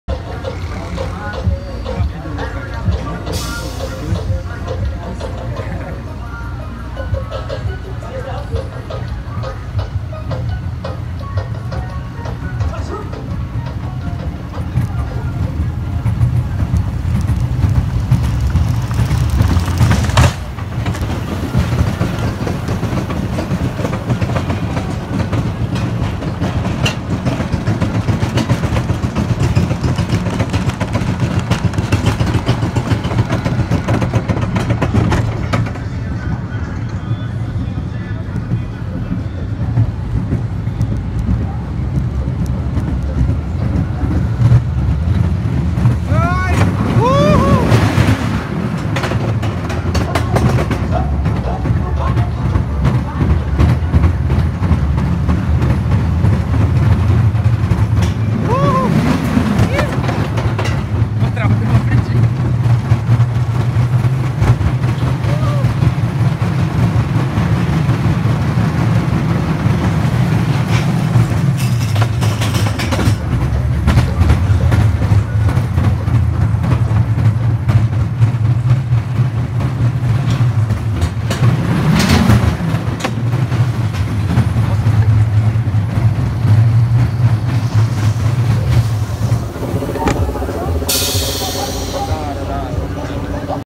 MONTANHA RUSSA (ROCK'N ROLL) - VITINHO PARK
Essa montanha-russa causa muito frio na barriga! A atração pertence ao parque itinerante Vitinho Park.